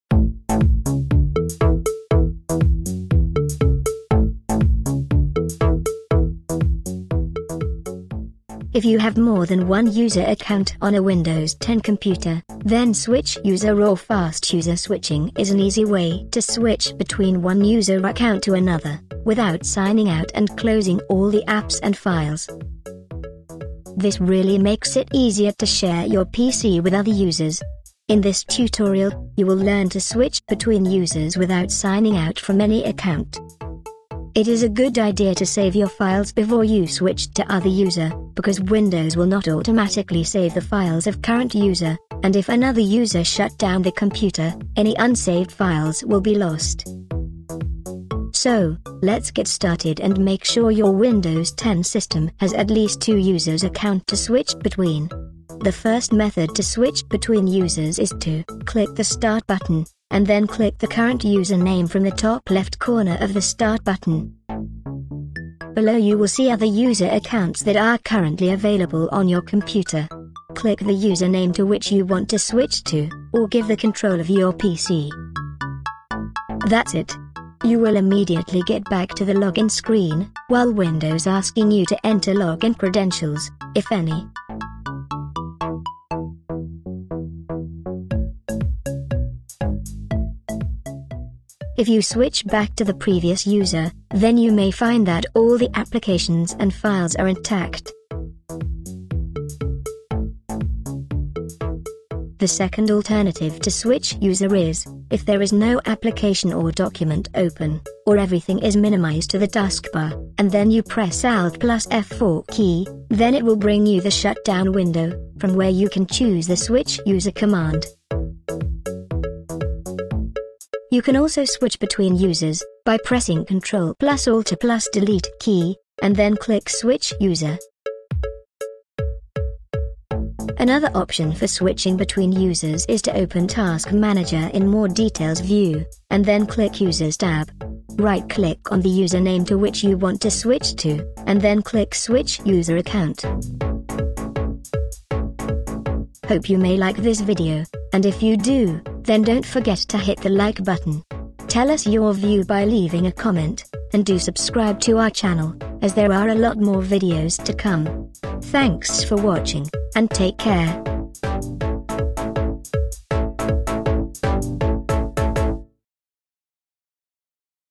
How to Switch Between Users Accounts on Windows 10 Tutorial | The Teacher
Switch Users or Fast User Switching in Microsoft Windows 10 & 8.1 is a feature that you can use to switch to another user without logging off from the current user. The benefit of using Switch User is that the current user does not need to close apps and documents before switching to another user. Switch User was first introduced with Windows XP and continues to support in the following version of Windows. Few methods describe in this video can also be used in other versions, Windows 7 and Windows XP.

Narrated By:
Emma (TTS)